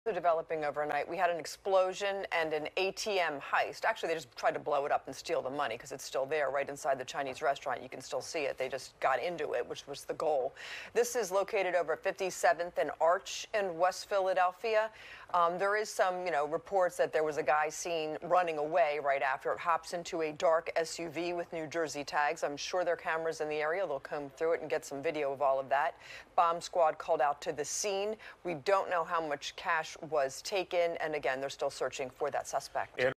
ATM blown up in brazen theft inside West Philadelphia restaurant: police
Police say a male suspect was seen fleeing with others in a dark SUV with New Jersey tags.